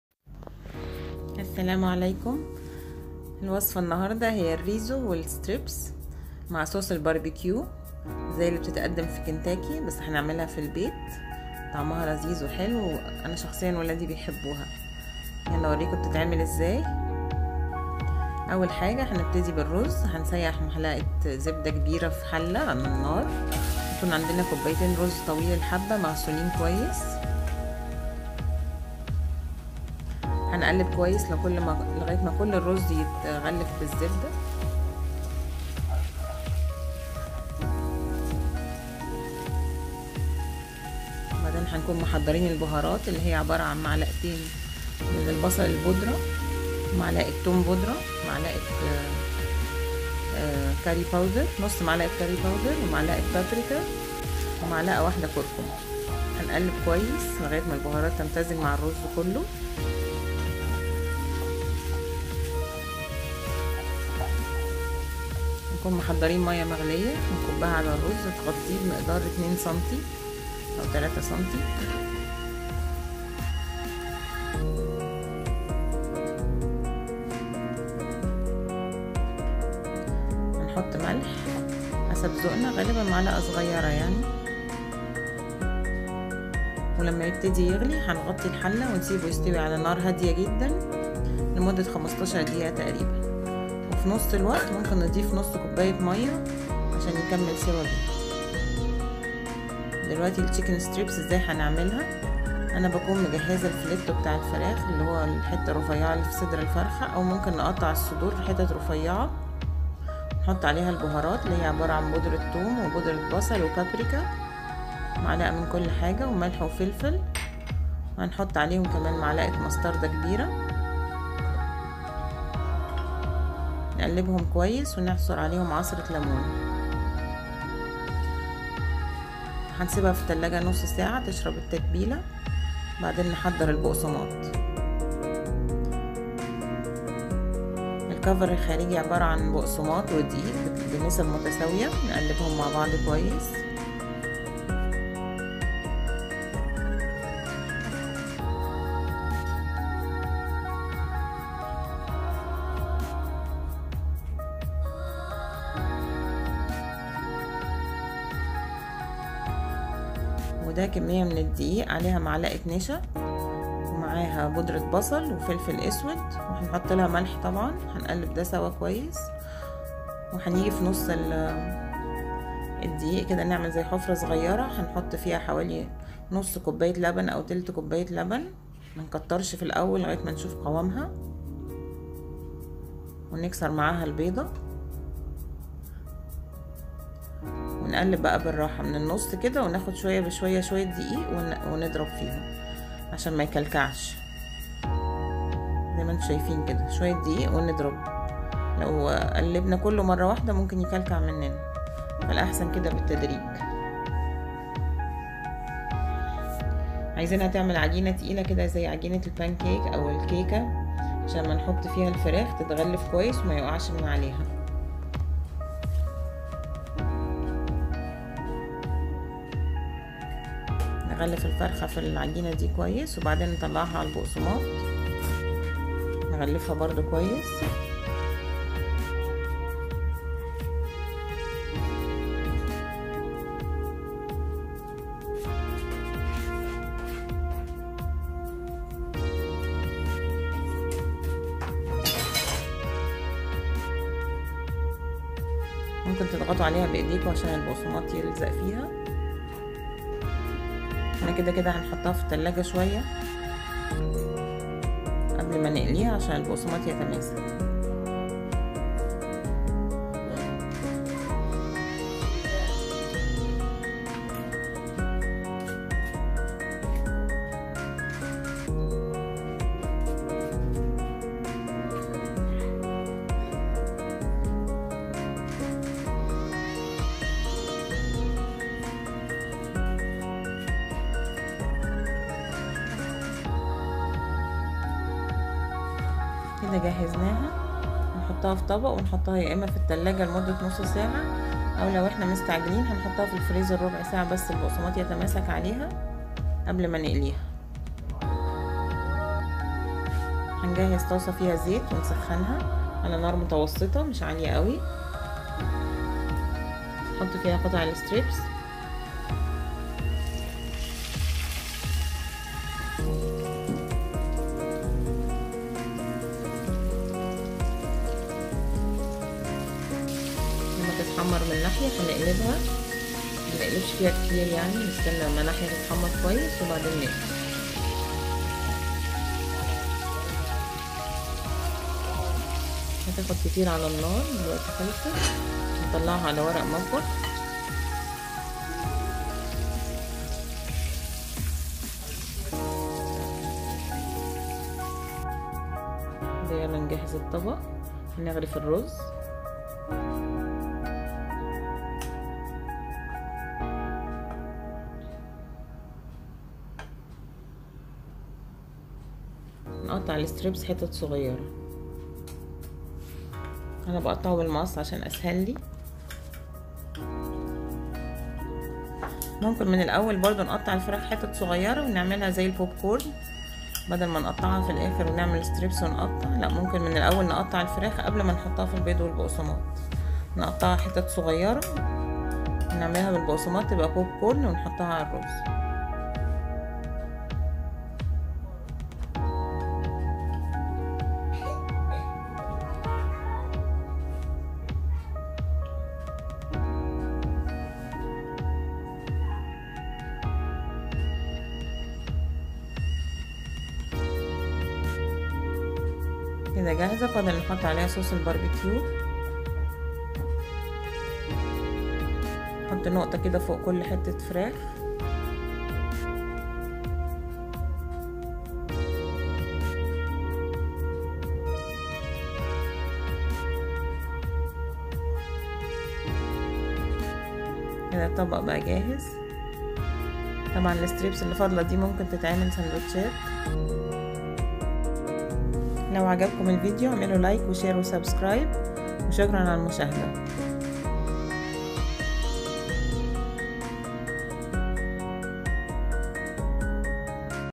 ريزو كنتاكي بقطع الدجاج وصوص الباربيكيو /Kentucky style Rizo& chicken
رجاء  الاشتراك فى القناة و تفعيل الجرس
المقادير
٢ كوب  أرز طويل الحبه
٢م ص بصل بودر
١ م ص ثوم بودر
١ م ص  كركم
١ م ص بامريكا
١/٢ م ص كاري
للدجاج(لكل نصف كجم)
م ص بصل بودر
م ص ثوم بودر
م ص بامريكا
ملح وفلفل
م ك مسترده
عصرة ليمون
 الغلاف الخارجي
 العجينة:
دقيق/ م ك نشا/ م ك بصل بودر/ فلفل / ملح
نصف كوب لبن / بيضه
بقسماط مطحون و دقيق